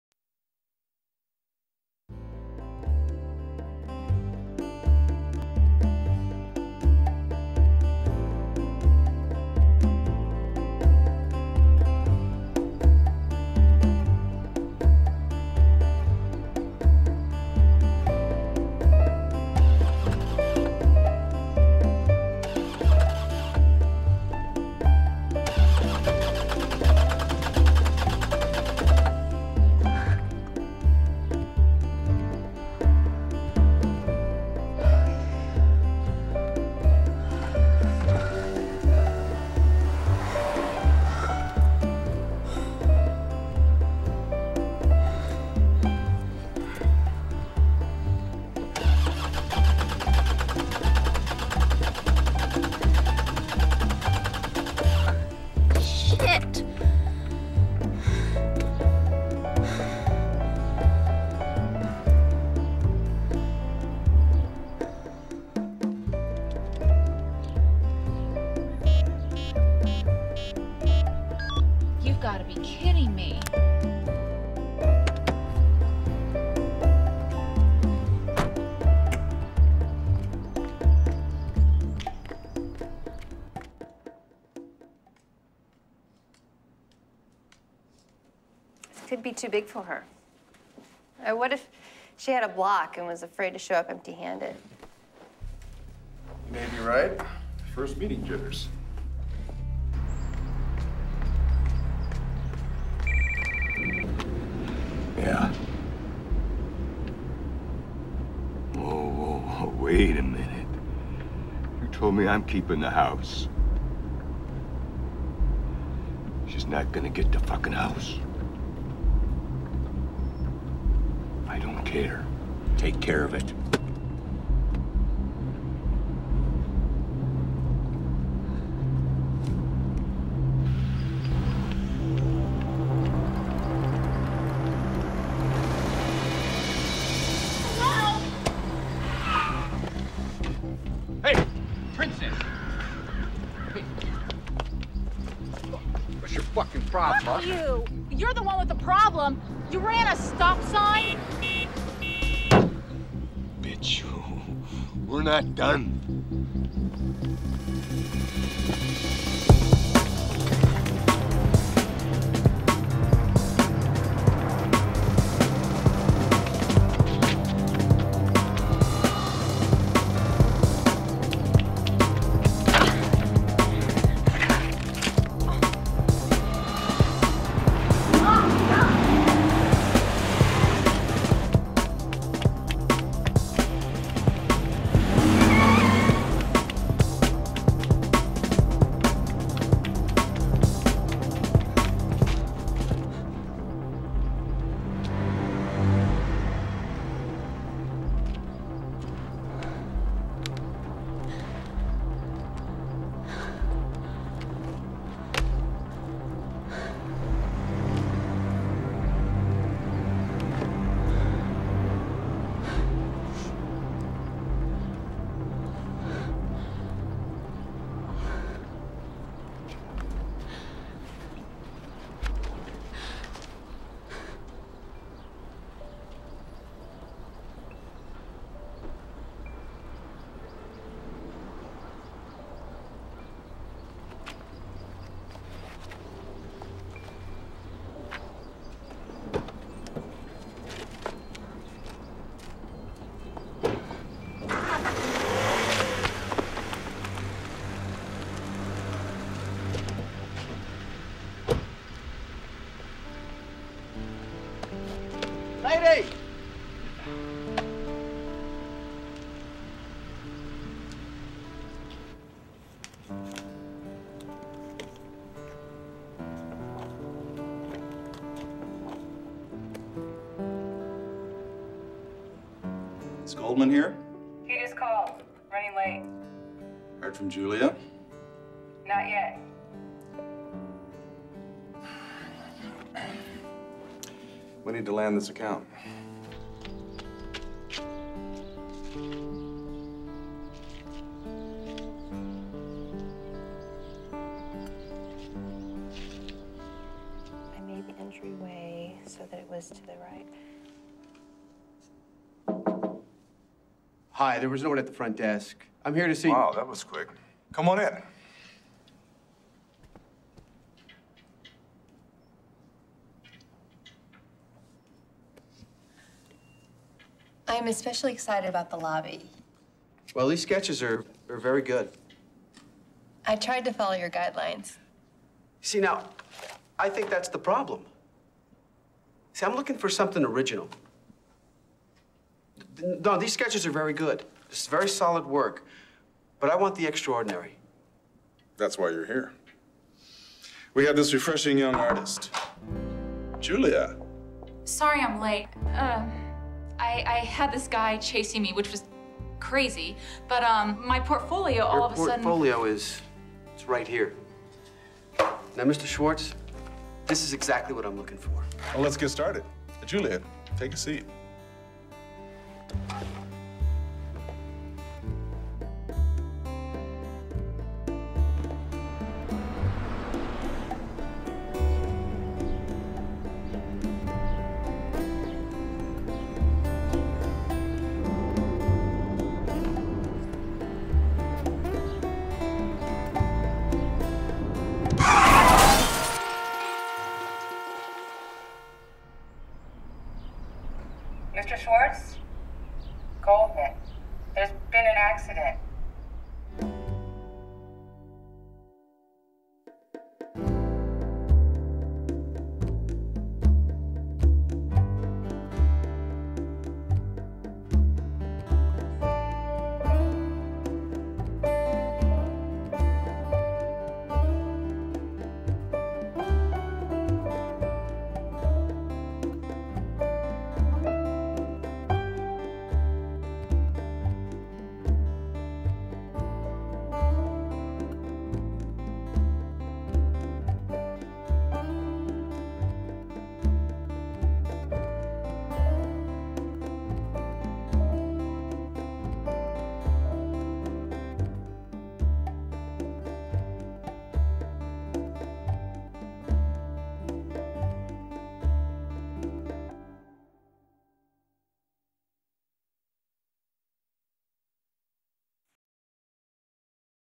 The Meeting
Julia is on her way to her first important meeting when her car breaks down. She takes off on foot and crosses paths with an angry driver who causes her to lose her portfolio. Convinced she's lost her shot at success, she's helped by a mysterious stranger.